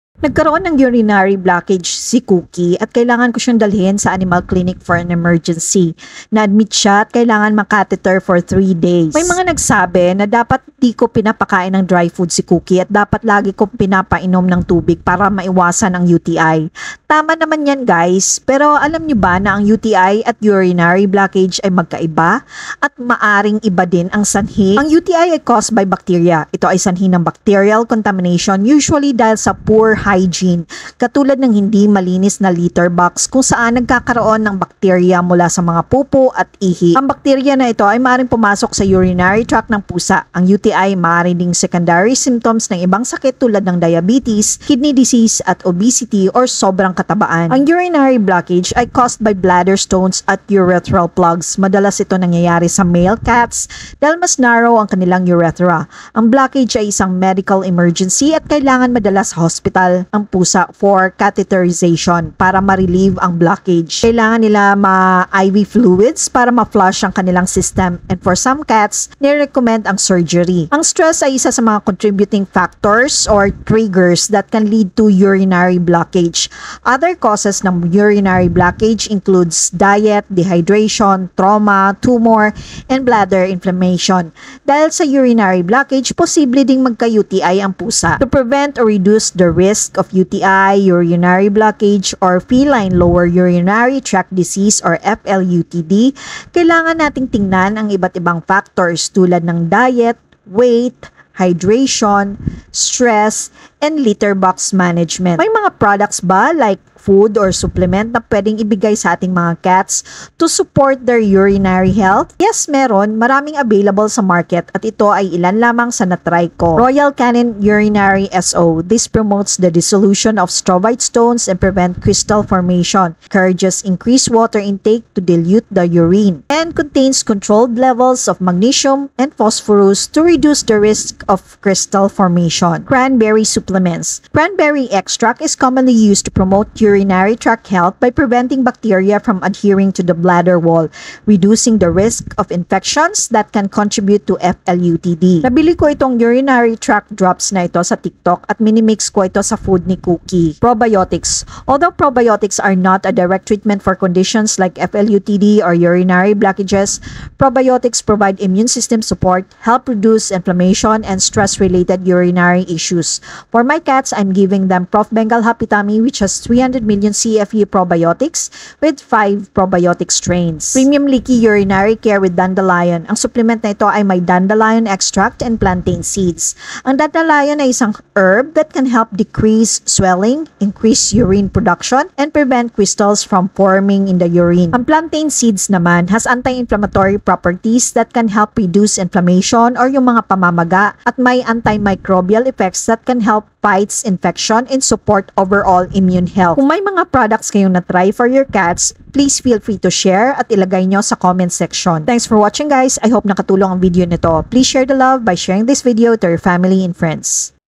UTI VS Urinary Blockage sa Pusa
Sa video na eto, pag uusapan nating ang tunglol sa UTI at Urinary Blcckage.

- Ano ang pinagkakaiba ng UTI at Urinary Blockage.
- Ano ang mga factors na dapat tingnan para ma prevent or mareduce ang mga Feline Lower Urinary Tract Diseases.
- Ano ang mga products or supplements na maaaring makatulong sa ating mga alaga to support their urinary health